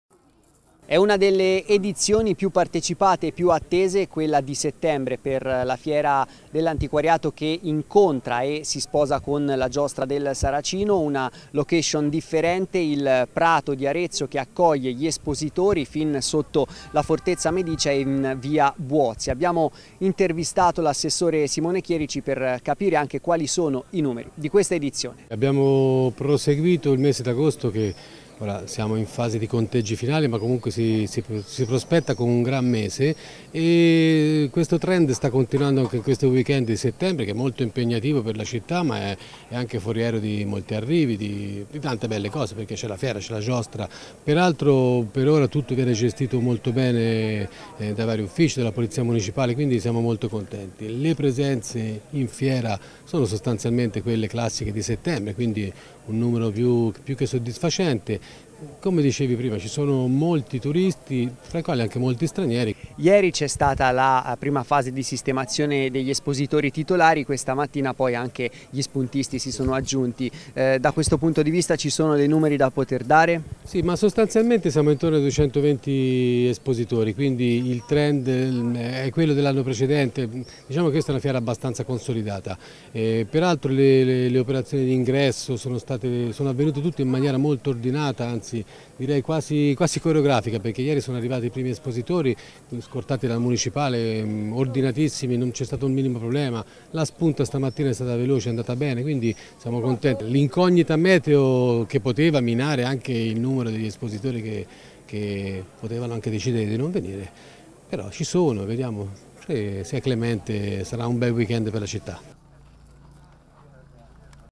La Fiera abbraccia la Giostra con l'incognita meteo
Per l'edizione di settembre la Fiera Antiquaria si sposta, come di consueto, al Prato.